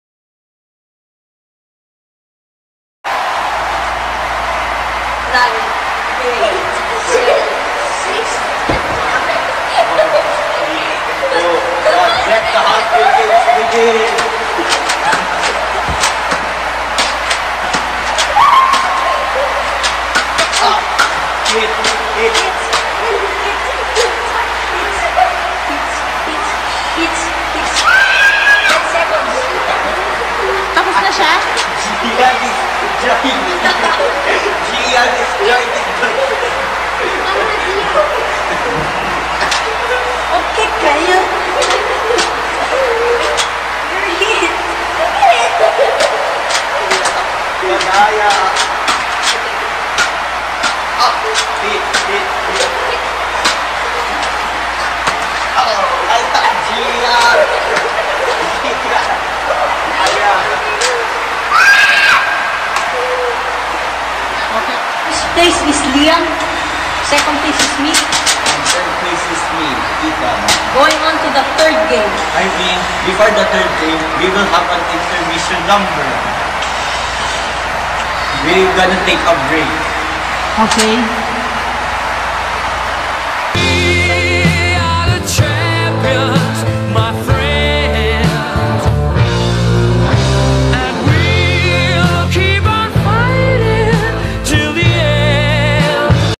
Nerf War goes free for all: Hungry James 2019
TAPED December 26, 2019. Witness the athletes shooting around one-on-one, free-for-all in the second game of the Hungry James, Nerf War aka Call of Duty IN REAL LIFE! Plus an unexpected surprise from one of our guests!